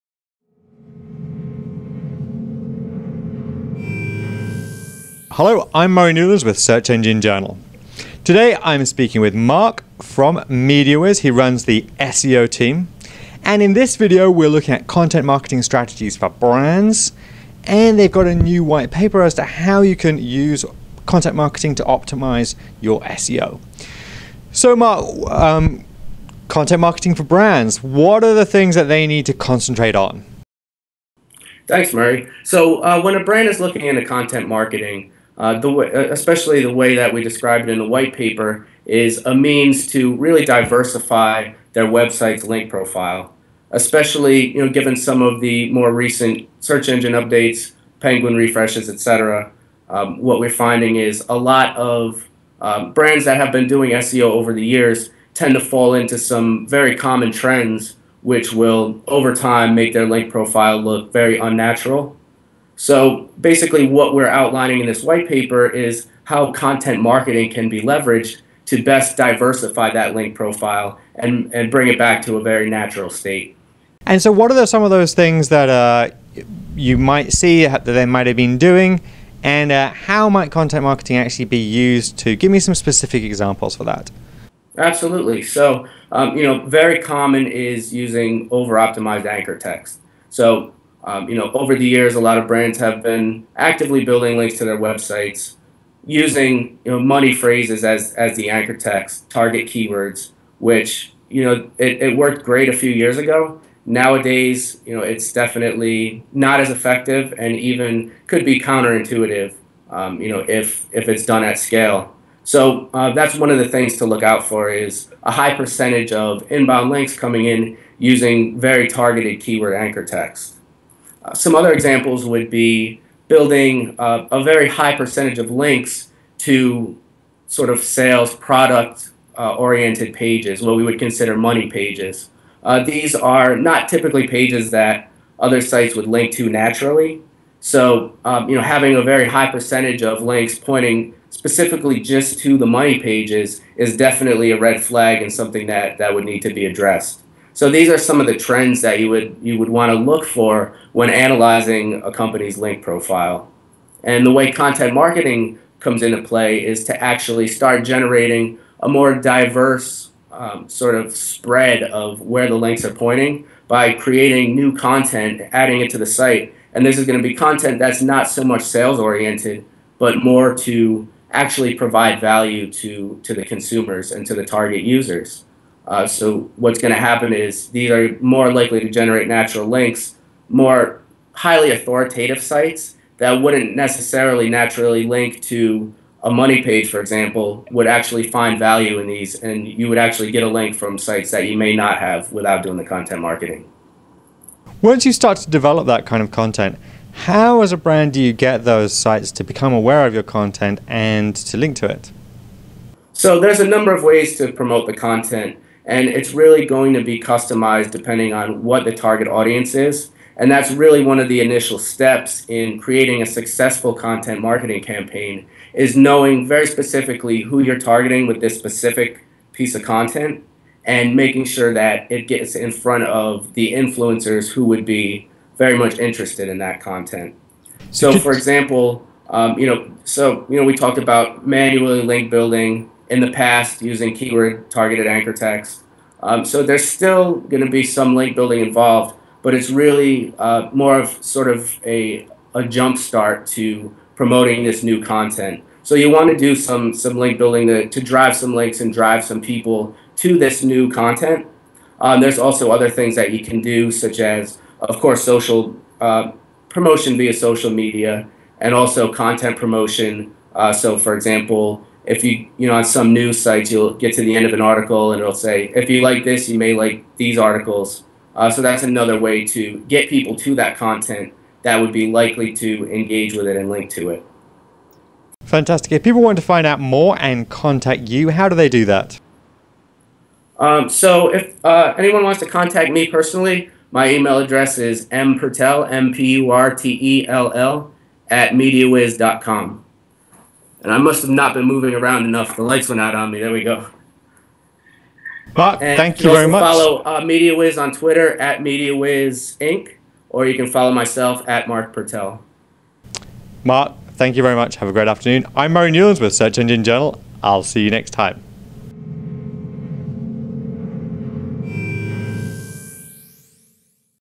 Content Marketing Strategies for Brands—An Interview with Marc Purtell
Search Engine Journal's Murray Newlands interviews Marc Purtell of MediaWhiz about content marketing, and the company's latest whitepaper.